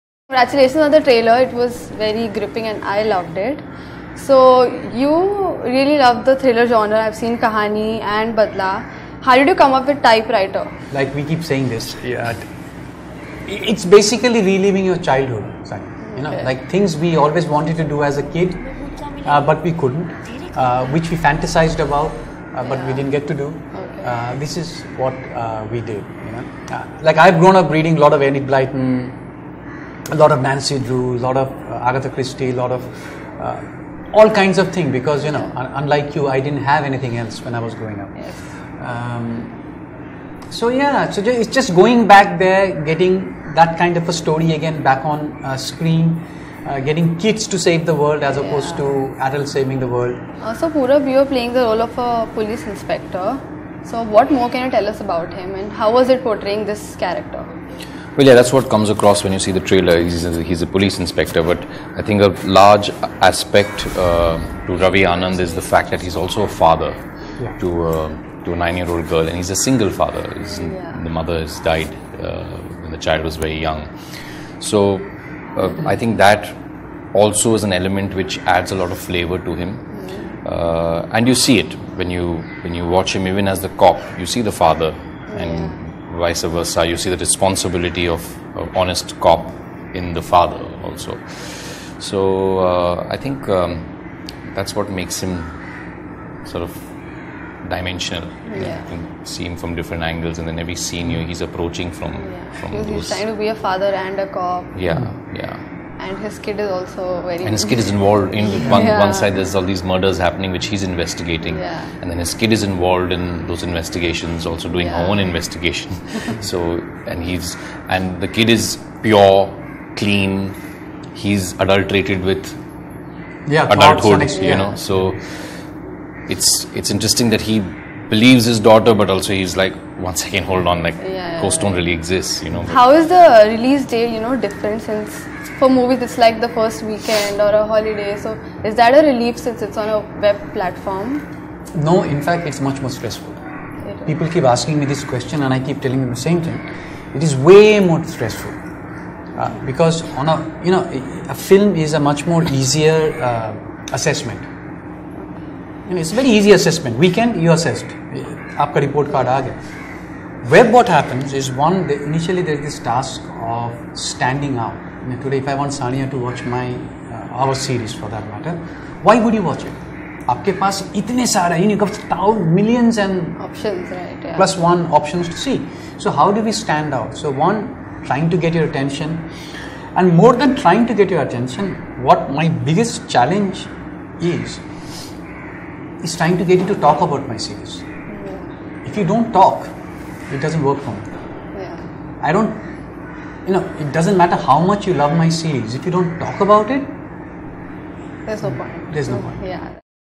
Sujoy Ghosh And Purab Kohli On Their Web Series 'Typewriter'
Filmmaker Sujoy Ghosh and actor Purab Kohli talk about their Netflix show 'Typewriter'. Sujoy Ghosh also tells us why a web release is more difficult as compared to a theatrical one.